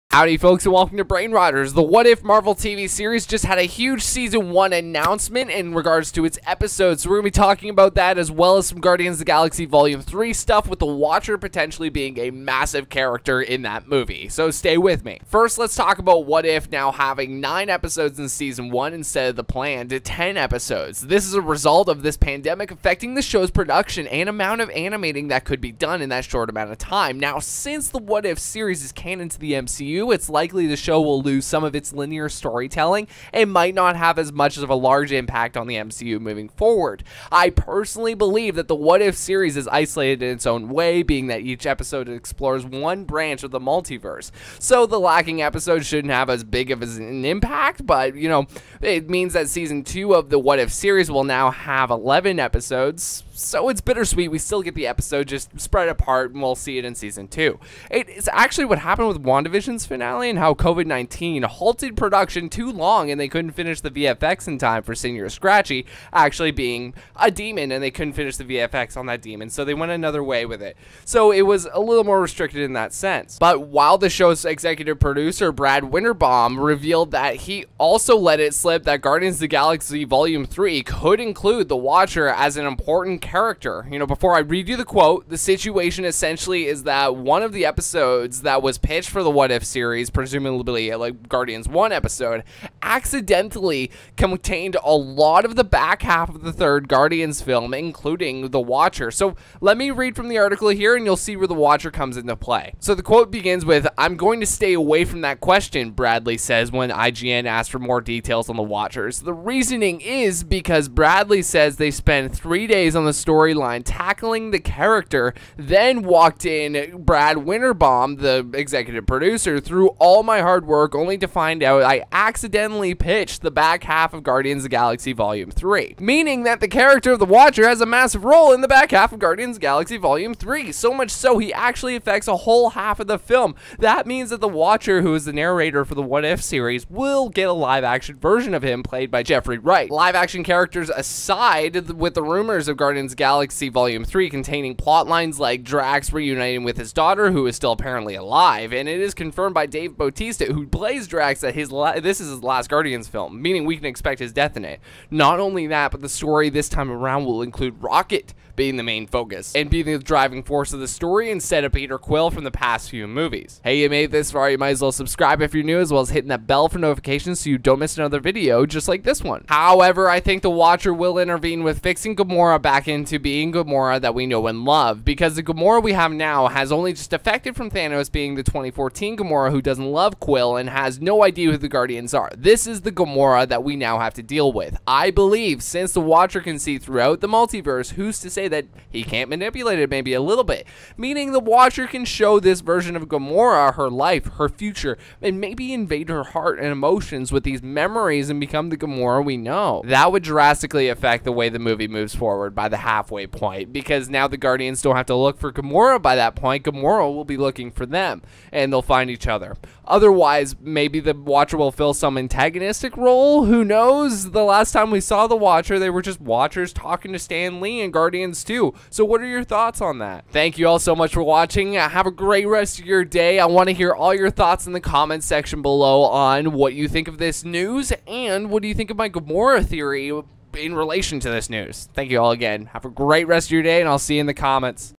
Marvel What If DELETED EPISODE Explained + Guardians Of The Galaxy Vol 3 Includes The Watcher!
The What If marvel series just got a whole lot of news leaked today including things on season 1’s episode count, and how Guardians Of The galaxy vol 3 might include the watcher in a big part of it.

First, let’s talk about What If having now 9 episodes in season 1 instead of the planned 10 episodes. This is a result of this pandemic affecting the show’s production and amount of animating that could be done. Now, since the What if series is canon to the MCu, it’s likely the show will lose some of its linear storytelling and might not have as large of an impact on the MCU moving forward. I personally believe the What If series is isolated in its own way being that each episode explores one branch of the multiverse, so the lacking episode shouldn’t have as big of an impact. It means that Season 2 of the What If series now will have 11 episodes; so it’s bittersweet. It’s actually what happened with WandaVision’s finale and how COVID 19 halted production too long and they couldn’t finish the VFX in time for senior scratchy actually being a demon. So, it was a little more restricted in that sense.
But, while the show’s executive producer, Brad Winderbaum, revealed that, he also let it slip that Guardians Of The Galaxy Vol 3 could include the Watcher as an important character. Before I read you the quote, the situation essentially is that one of the episode’s he pitched for the What If series, presumably a Guardians 1 episode, accidentally contained a lot of the back half of the third Guardians film. So, let me read from the article here and you’ll see where the watcher comes into play, ‘“I'm going to stay away from that question," Bradley says when IGN asked for more details on the Watchers. The reasoning is because Bradley says they spent three days on a storyline tackling the character then, "walked [executive producer] Brad Winderbaum through all my hard work only to find out I accidentally pitched the back half of [Guardians of the Galaxy vol. 3].”’
Meaning, that the character of the Watcher has a massive role in the back half of Guardians Vol 3. So much so, he actually affects a whole half of the film. That means that the Watcher, who is the narrator for the What If series, we’ll get a live action version of him played by Jeffrey Wright. Live-action characters aside, with the rumours of Guardians 3 containing plotlines like Drax reuniting with his daughter who is still apparently alive, and it is confirmed by Dave Bautista who plays Drax that it’s his last guardians film, meaning we can expect his death in it. Not only that, but the story this time around will include Rocket being the main focus of the team instead of Peter Quill from the past few movies. However, I think the Watcher will intervene with fixing Gamroa back into being the Gamora we know and love. Because the gamora we have now, has only just defected from Thanos, being the 2014 Gamora who doesn’t love Quill and has no idea who the Guardians are. I believe, since the Watcher can see throughout the multiverse, who’s to say he can’t manipulate it a bit? Meaning, the Watcher can show this version of Gamora her life, her future, and maybe invade her heart and emotions with these memories and become the Gamora we know. That would drastically affect the way the movie moves forward by the halfway point, because now the guardians don’t have to look for Gamora by that point. Otherwise, maybe the watcher will fill some antagonistic role. Who knows? The last time we saw the watcher, there were watchers talking to Stan Lee in Guardians 2. What’re your thoughts?